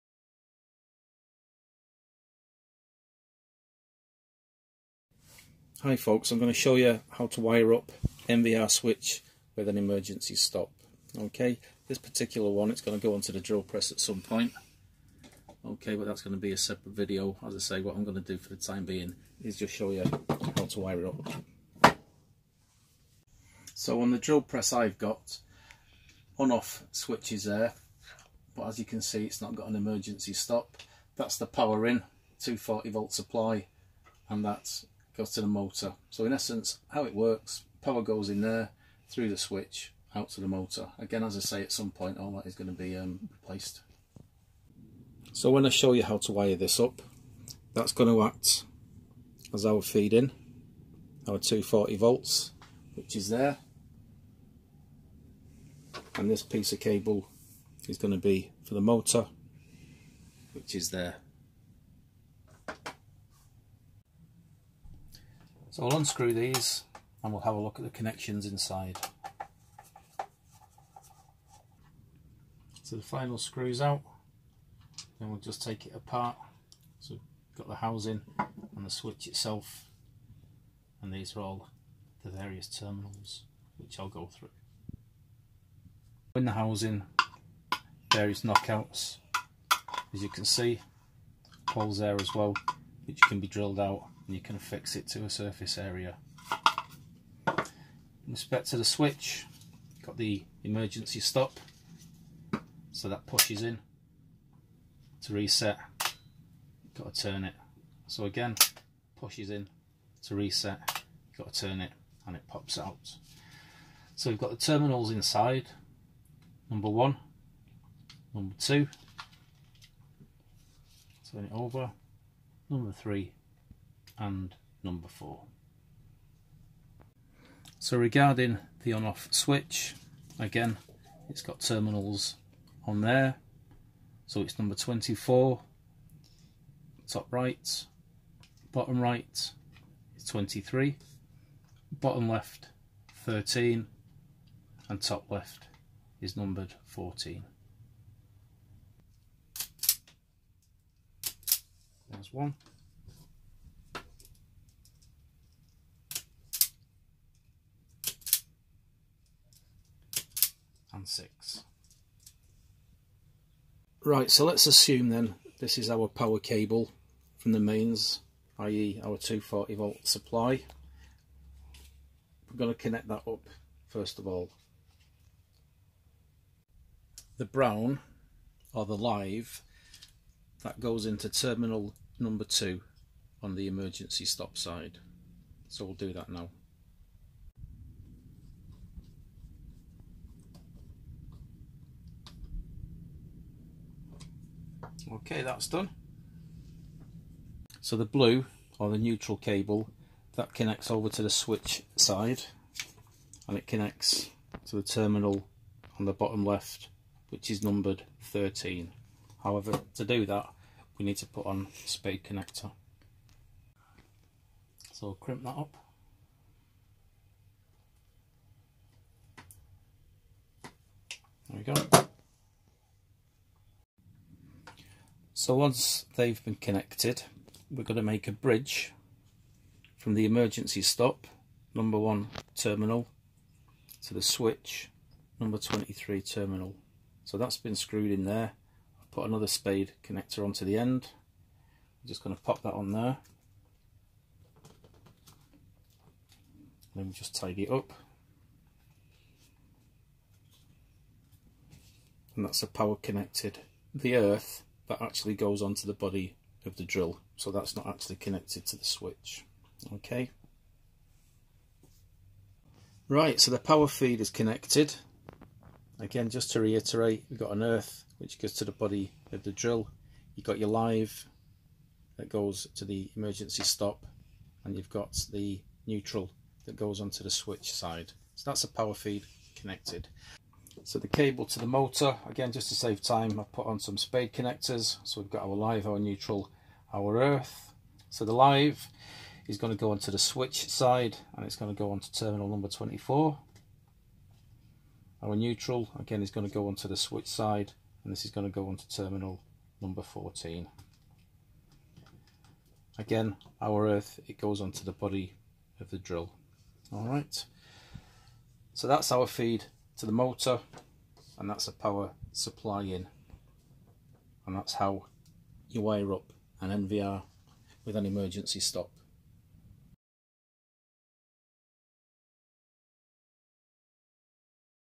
How to Wire an NVR Switch with Emergency Stop
A quick video on how to wire an NVR switch ( no-volt-release switch) with an emergency stop (e-stop).

Video intended as a guide only. If in any doubt consult a suitably qualified electrician.